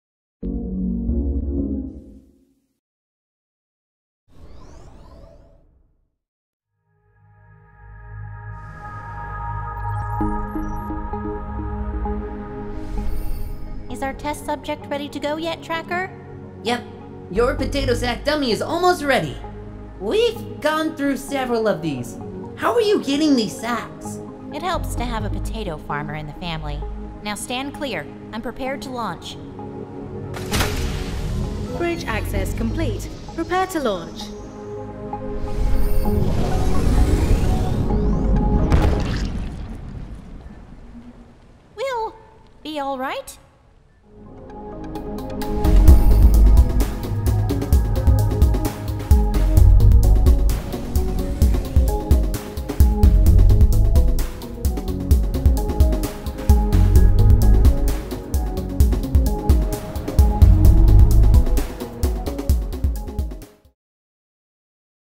Build of the Universe Machine Comic | Launch Day
Better safe than mashed potatoes.

The comics are refurbished -- now with voice acting! Please enjoy the motion comics, there will be more soon!

Please be sure to follow and thank the extremely talented cast:

Jenna Harley - @MoonsquidInk
Tracker - @voiceofoboyle
A.N.N.I. - @synaes